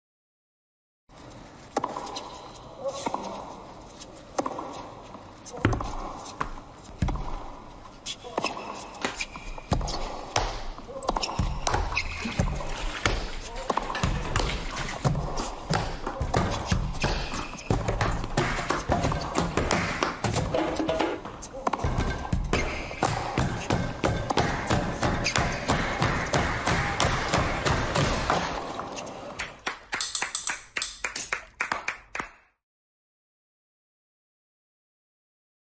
STOMP Creates Piece Inspired by US Open
From BroadwayWorld's Archive: Watch STOMP perform a new piece inspired by the US Open!